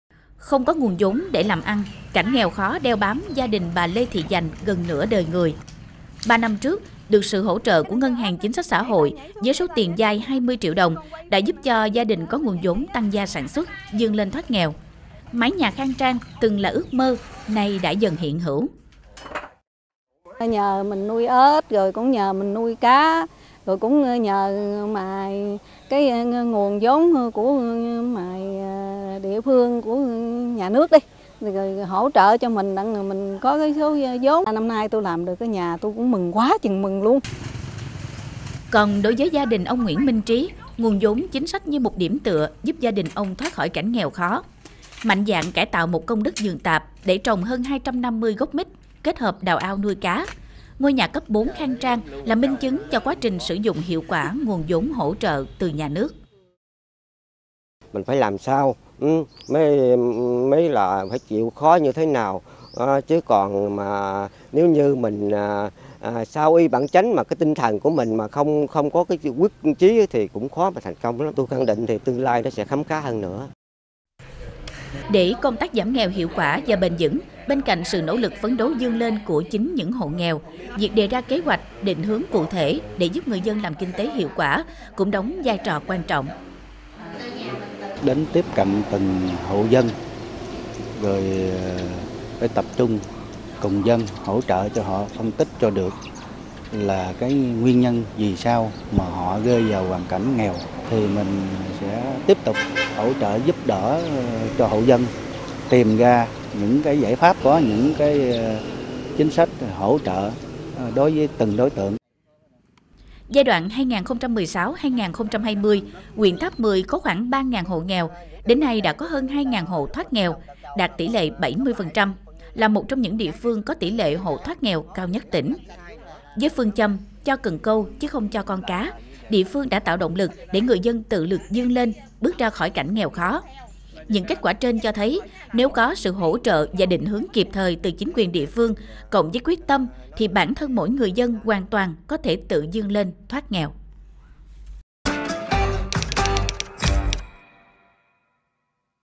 NỖ LỰC TỰ VƯƠN LÊN THOÁT NGHÈO | VTV5
Để công tác giảm nghèo hiệu quả và bền vững, bên cạnh sự nỗ lực phấn đấu vươn lên của chính những hộ nghèo, việc đề ra kế hoạch, định hướng cụ thể để giúp người dân làm kinh tế hiệu quả cũng đóng vai trò quan trọng.